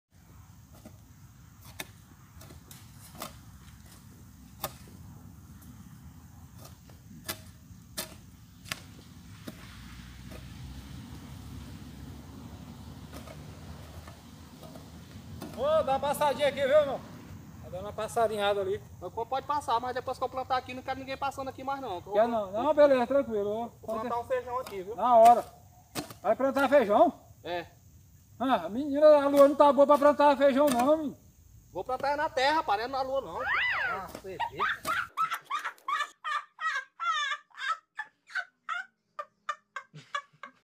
homem ignorante
Agricultor preparado a terra para o plantio de feijão, quando alguém. !